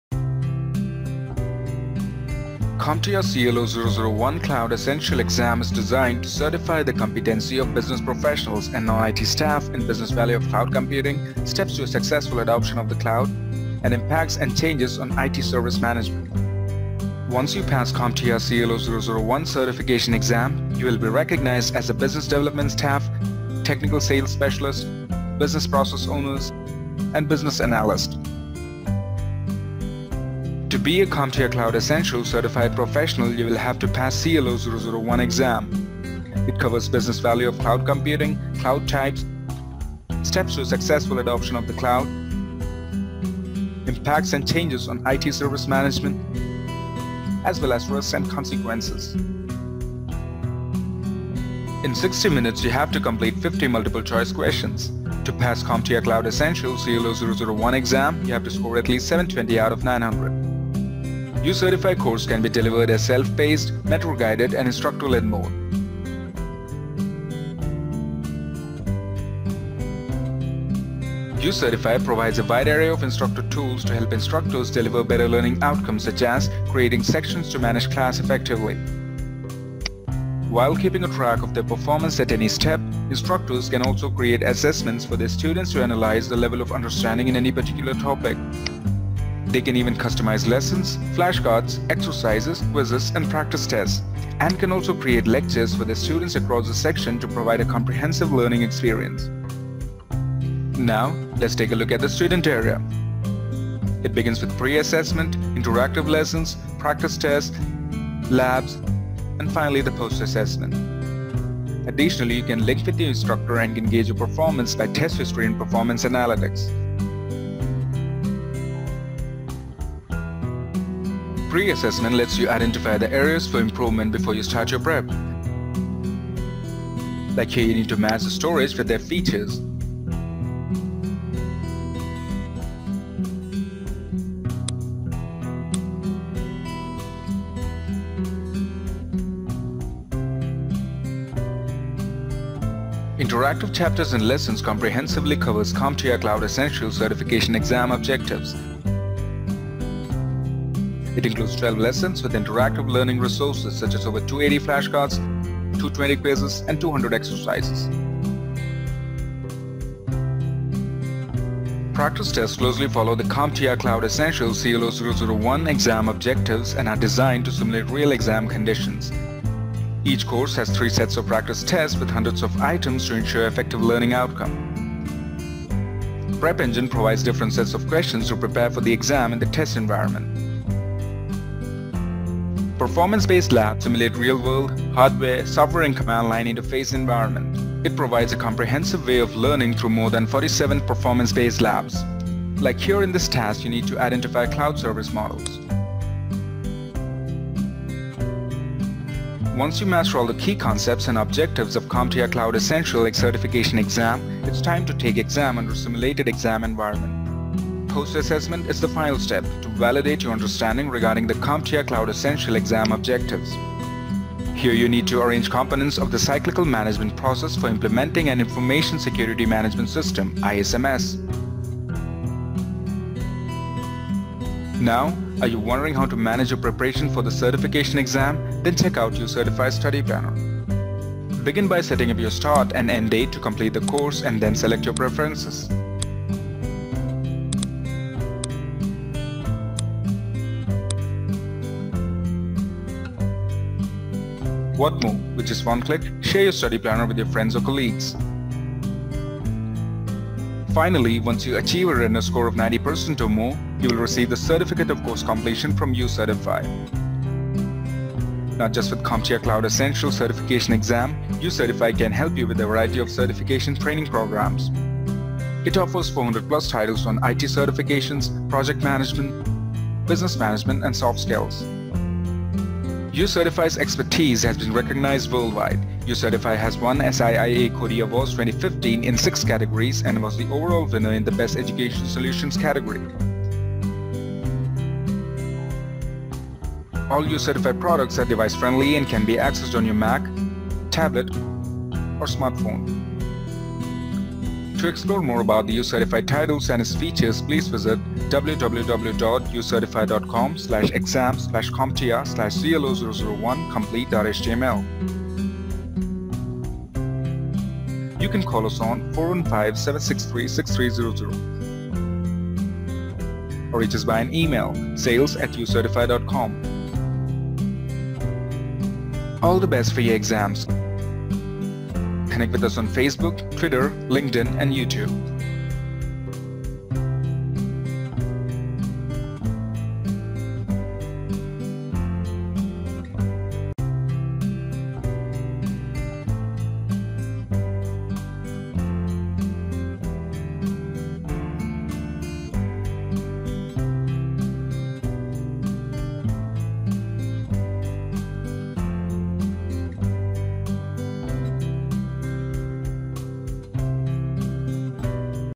CompTIA CLO-001 Complete (Course & labs)
Kick start your prep for CompTIA CLO-001 certification with interactive lessons, test prep, and performance-based labs. Watch this video now to explore the product!